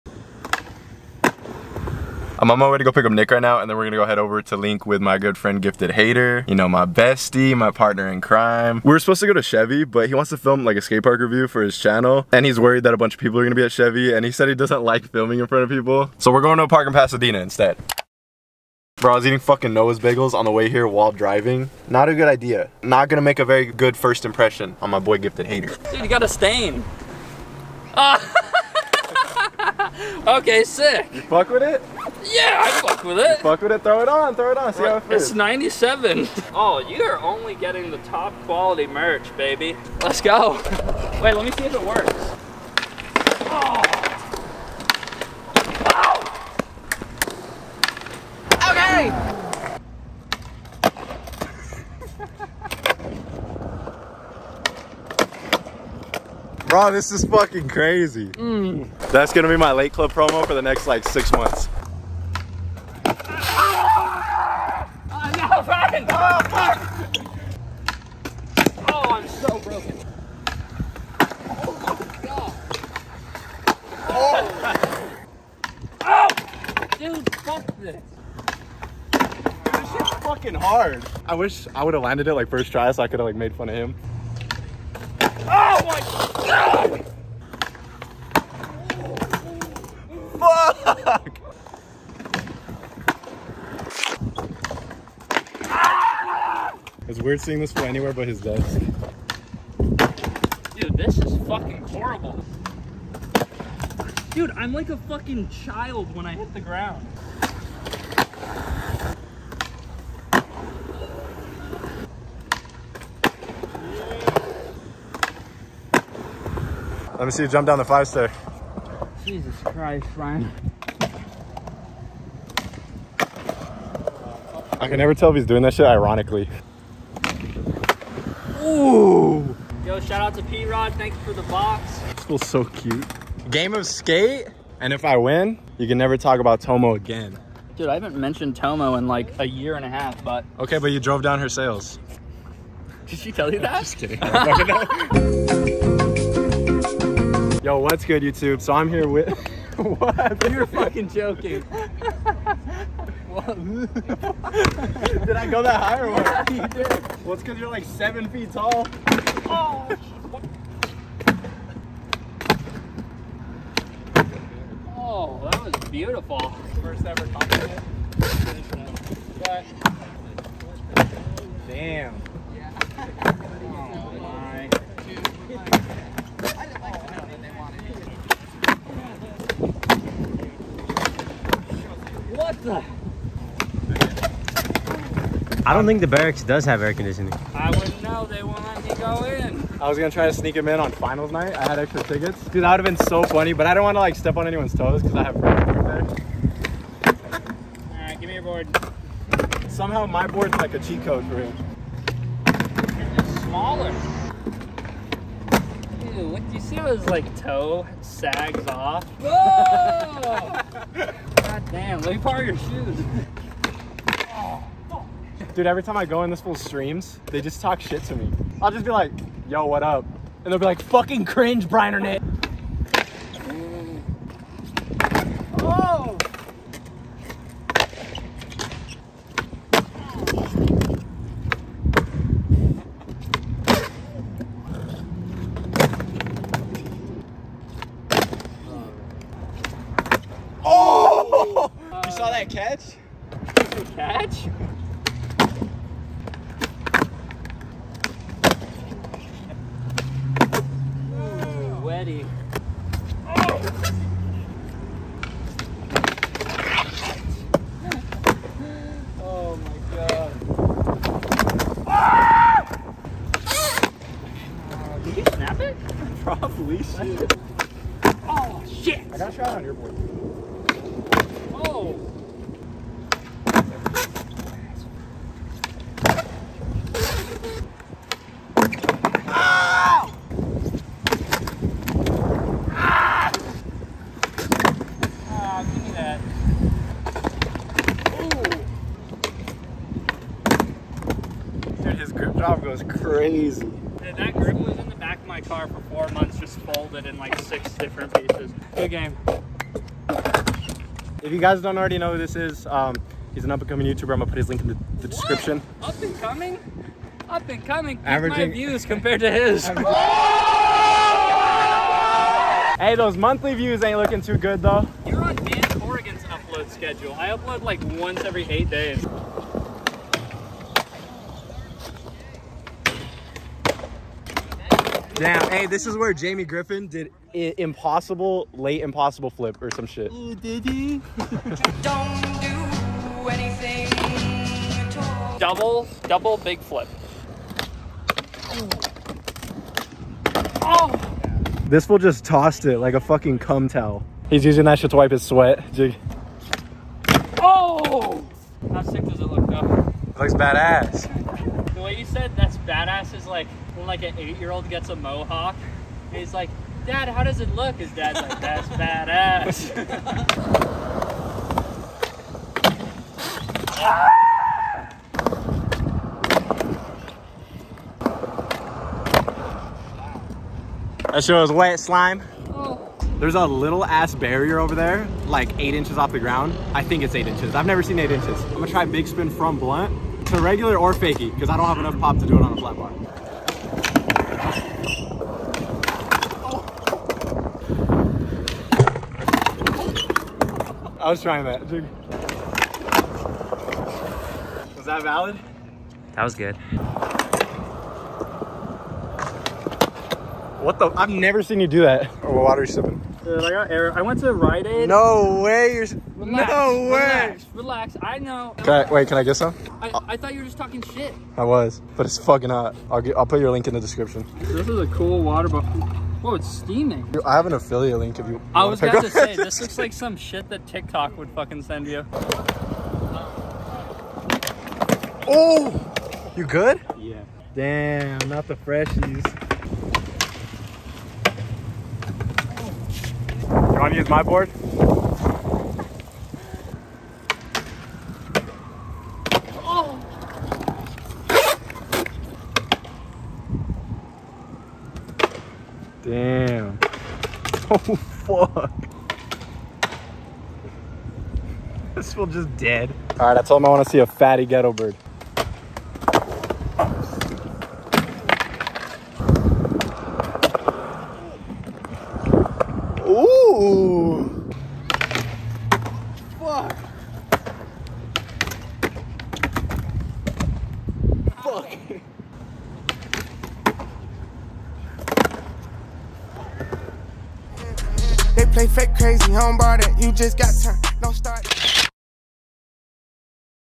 Yellow Skatepark with Gifted Hater!
Somehow convinced Gifted Hater to skate a yellow park lmao. Had a fun session in Pasadena. Hope you enjoy, love you guys!